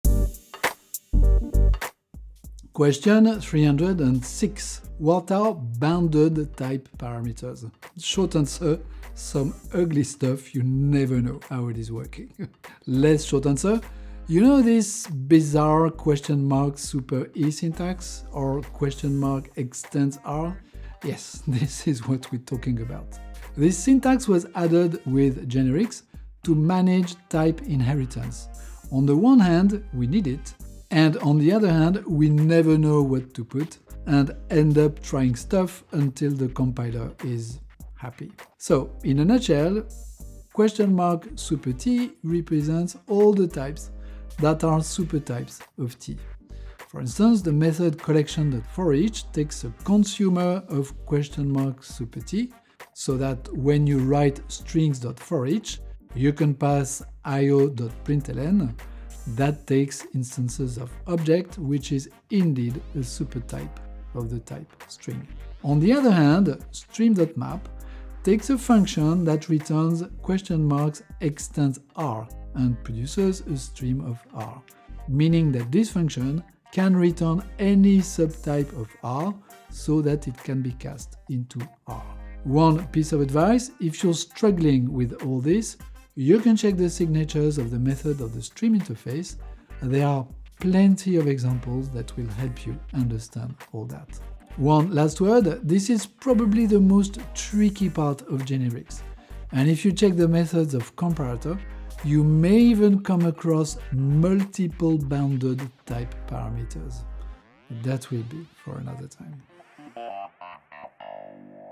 What are bounded type parameters? - Cracking the Java Coding Interview
Cracking the Java Coding Interview - Question 306: What are bounded type parameters?